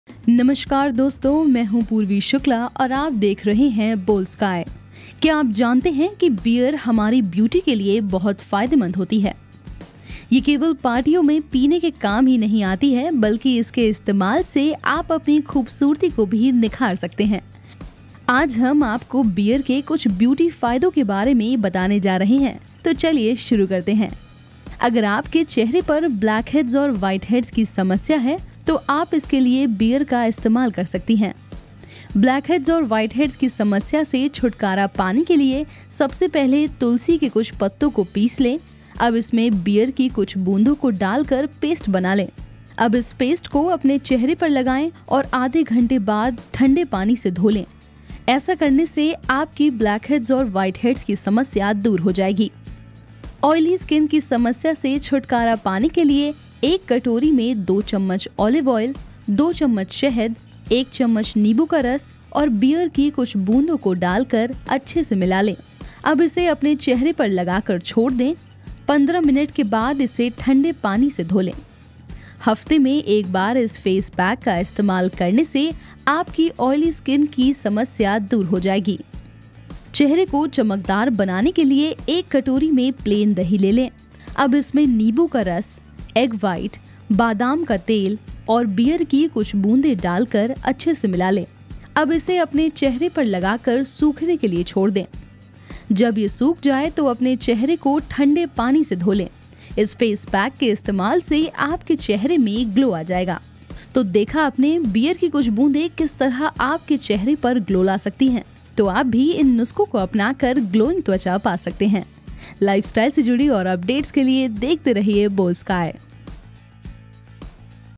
Beer Skincare | बीयर की कुछ बूंदे बनाएंगीं चेहरे को चमकदार | Boldsky
Beer is one alcoholic beverage that is used in the beauty regime for the hair and skin care. Beer is a soothing cleanser which helps to dissolve dead skincells and increase the elasticity of the skin. It contains B vitamins, hops, saccharides, and yeast which are beneficial to the skin in many ways.

बियर हमारी ब्यूटी के लिए बहुत फायदेमंद होती है. यह केवल पार्टियों में पीने के काम ही नहीं आती है बल्कि इसके इस्तेमाल से आप अपनी खूबसूरती को भी निखार सकते हैं. आज हम आपको बियर के कुछ ब्यूटी फायदों के बारे में बताने जा रहे हैं. तो चलिए शुरू करते हैं।